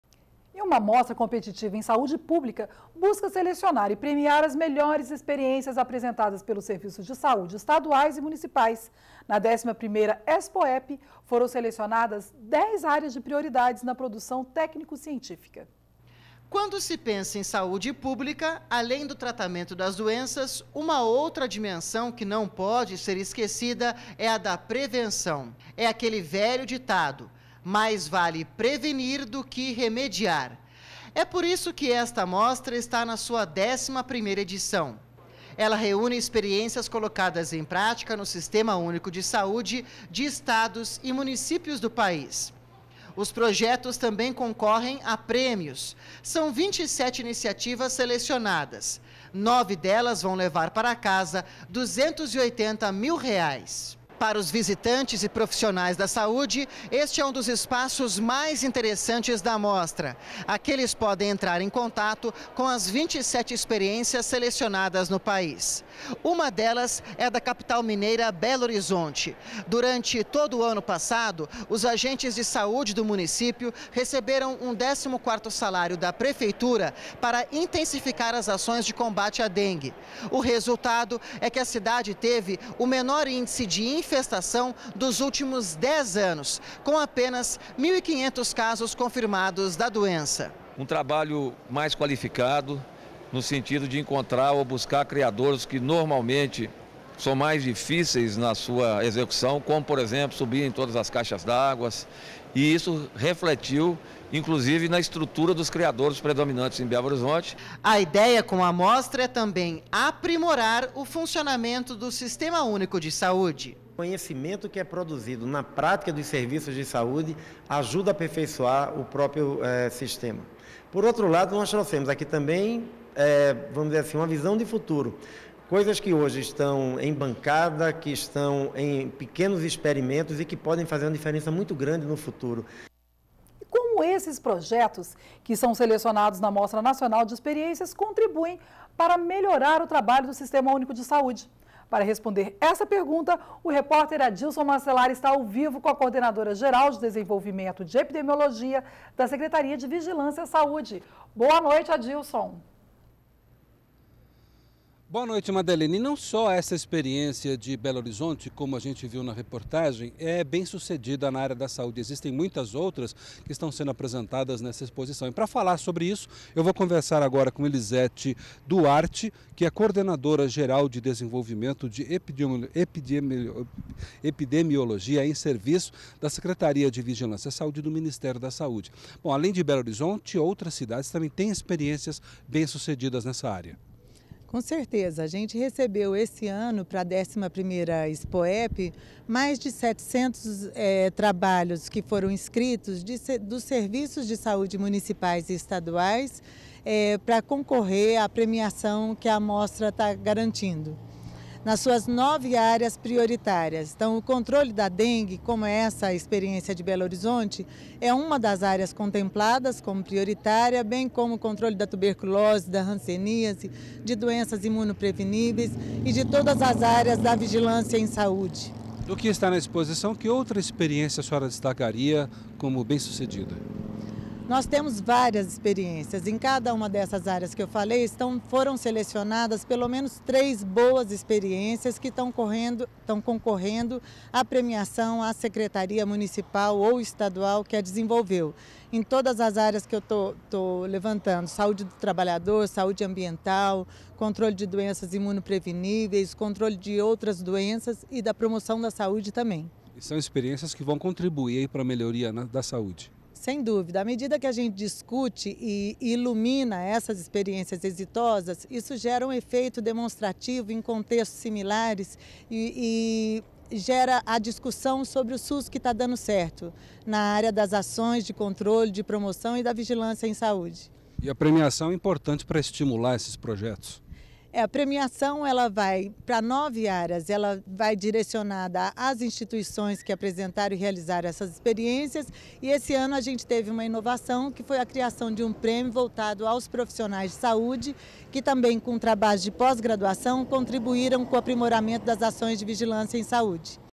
Melhores experiências em saúde pública são selecionadas e premiadas na 11ª EXPOEPI
NBR Notícias - 02.11.11: Uma Mostra Competitiva, em saúde pública, busca selecionar e premiar as melhores experiências apresentadas pelos serviços de saúde estaduais e municipais. Na 11ª EXPOEPI foram selecionadas 10 áreas de prioridades na produção técnico-científica. E como esses projetos, que são selecionados na Mostra Nacional de Experiências contribuem para melhorar o trabalho do Sistema Unico de Saúde?